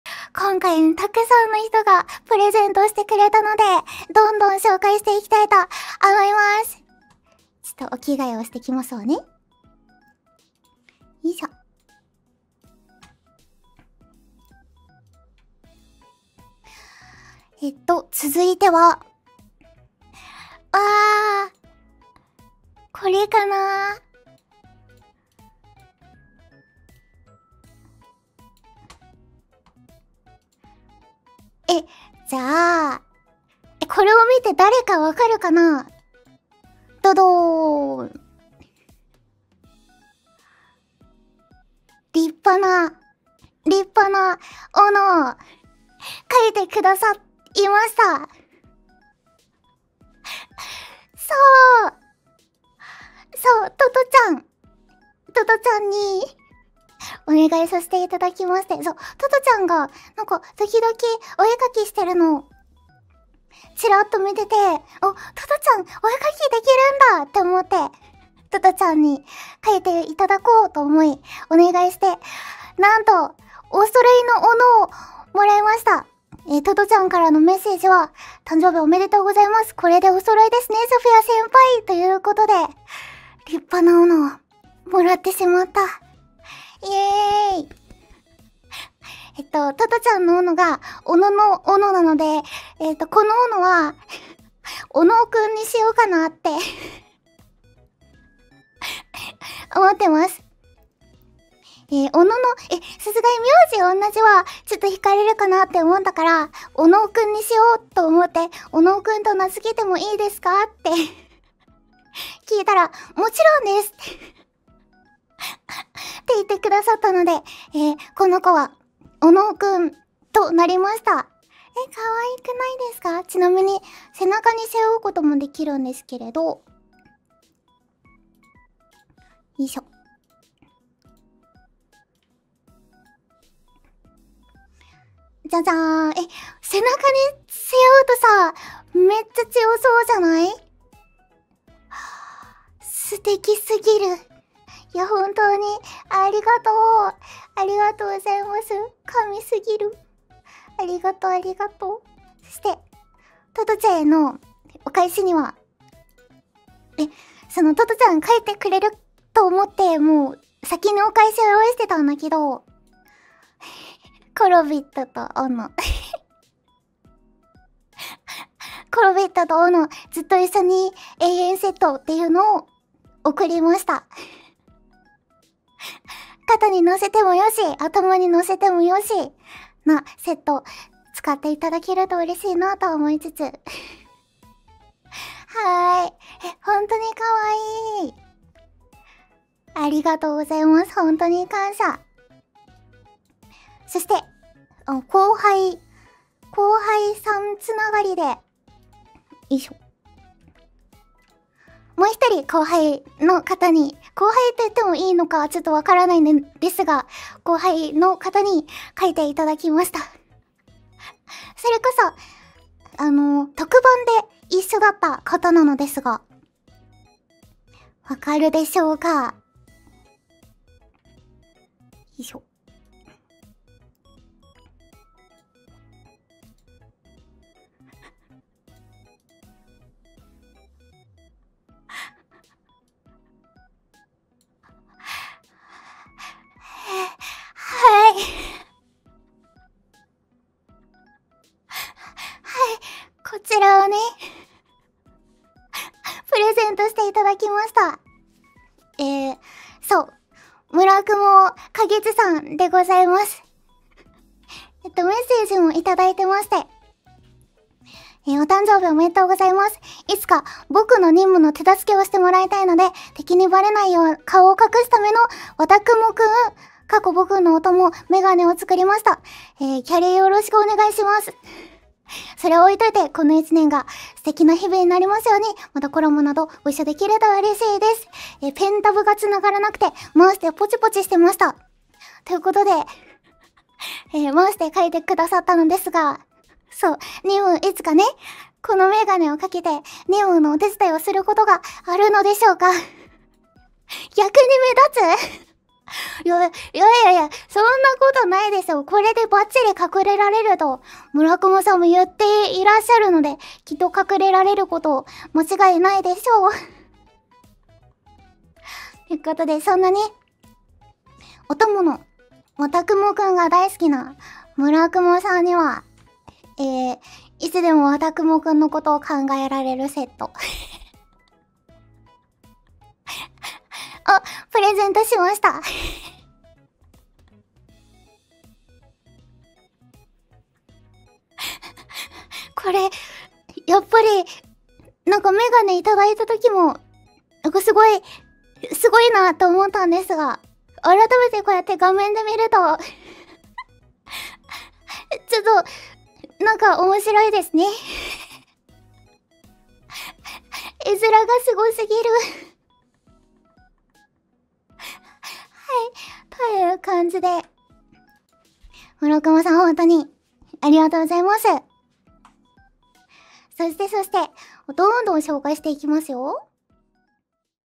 【誕生日プレゼント】カゲツくんと都々ちゃんにプレゼントを貰うソフィー【叢雲カゲツ/立伝都々/ソフィアバレンタイン/にじさんじ/新人ライバー】
◎配信元
【 ソフィアの誕生日2024 】🎂祝って！もらって！歌って！な誕生日記念配信🎉【にじさんじ /ソフィア・ヴァレンタイン】
ご連絡用X(旧Twitter) @sinnzinnnizi